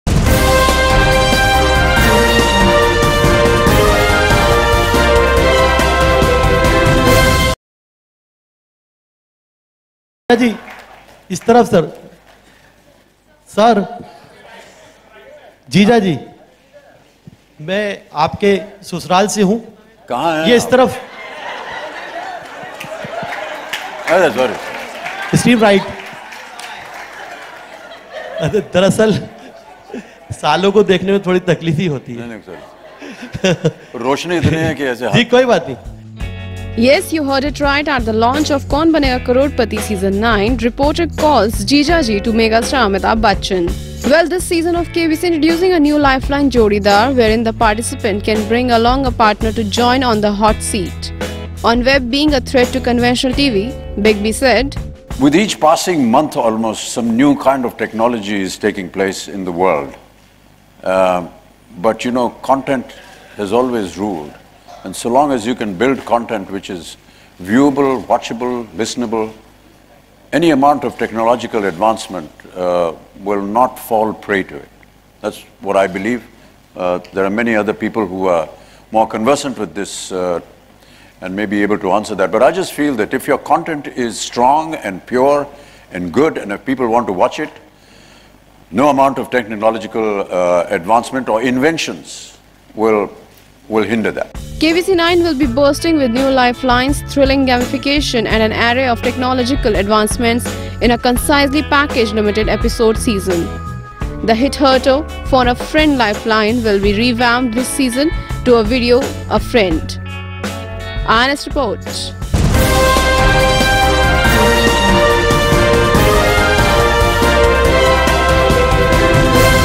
Funny Moment Amitabh Bachchan to Reporter Calls “JIJA JI”
Yes you heard it right, at the launch of "Kaun Banega Crorepati" (KBC) season 9, s reporter calls “Jija ji” to Megastar Amitabh Bachchan. Well, this season of KBC, introducing new life line "jodidaar" wherein the participant can bring along a partner to join on the hot seat.